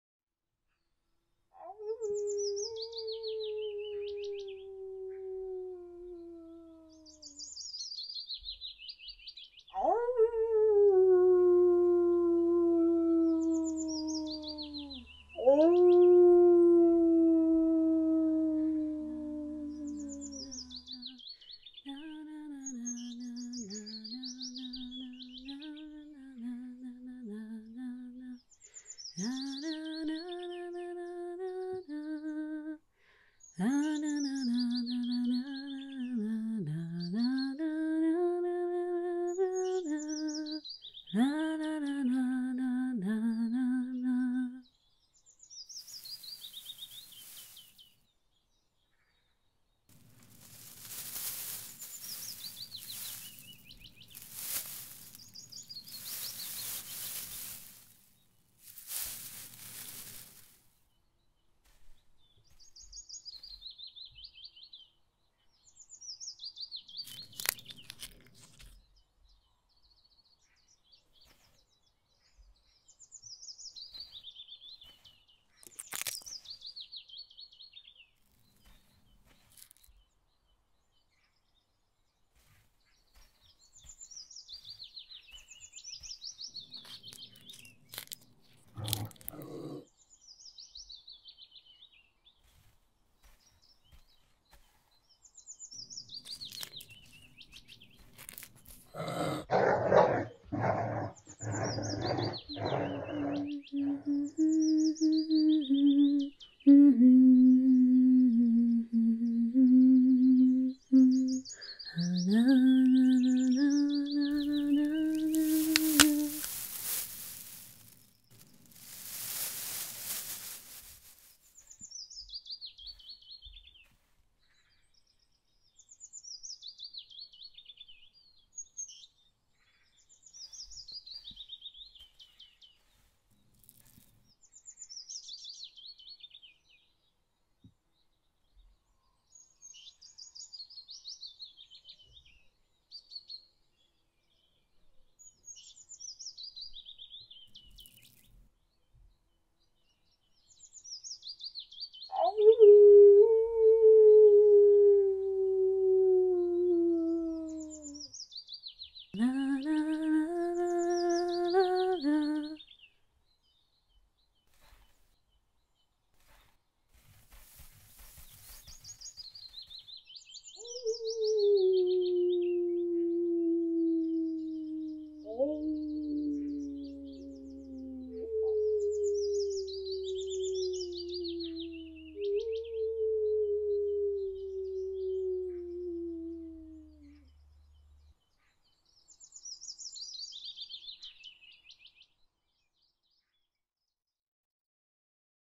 The Wolf & Princess Rags | CUTOUT SHORT FILM
This little cutout animation features Princess Rags, a character I'm currently trying to write a book about.
I made this film to explore a scenario that will happen in the story.